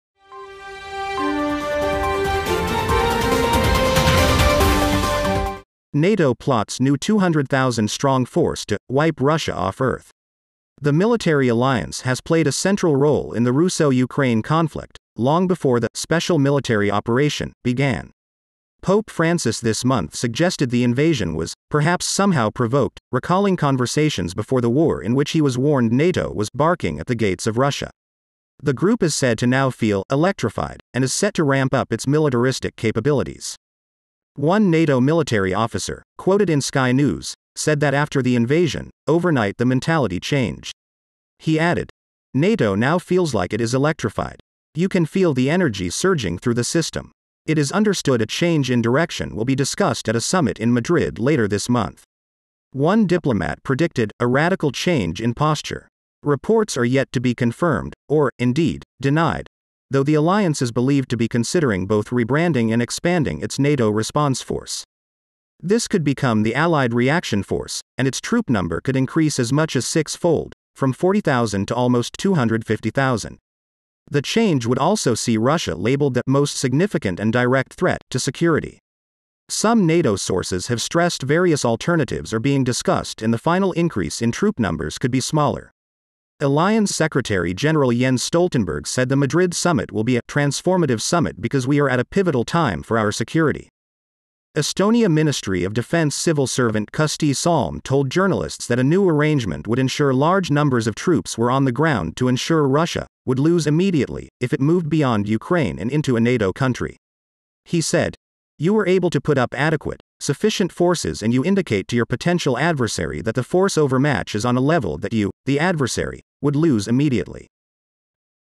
Putin's Nightmare: NATO Plots New 200,000 Strong Force to Wipe Russia Off Earth
The group is said to now feel “electrified” and is set to ramp up its militaristic capabilities. One diplomat predicted “a radical change in posture”.

This could become the Allied Reaction Force, and its troop number could increase as much as six-fold, from 40,000 to almost 250,000.

The change would also see Russia labelled the “most significant and direct threat” to security.